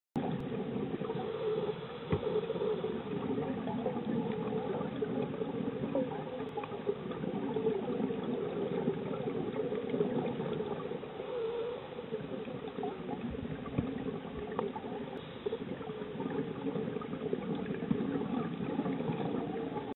Potato Bass at Doodles, Ponta do Ouro, Mozambique
Friendly potato bass at Doodles in Ponta do Ouro. Look out for the cleaner wrasses.

For more information on scuba diving in Southern Africa